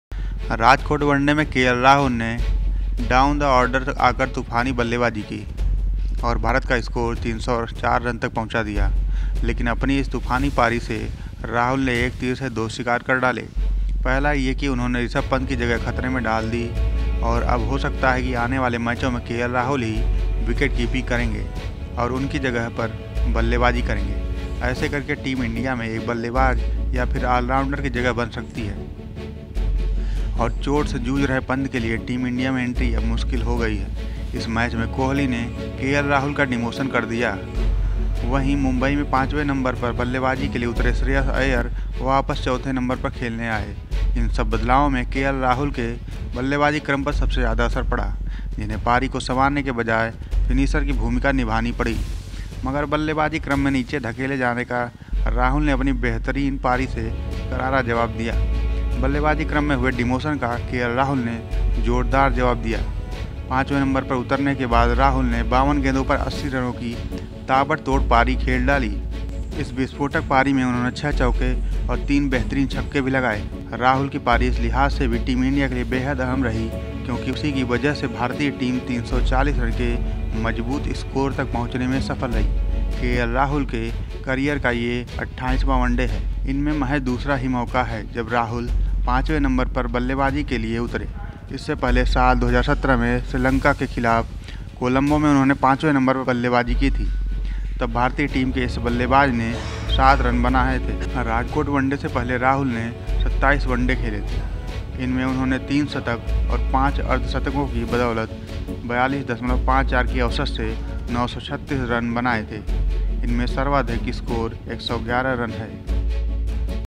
के एल राहुल छा गए।। राजकोट में
के एल राहुल छा गए।। राजकोट वनडे में राहुल की तूफानी पारी ने टीम में राहुल की जगह पक्की कर गई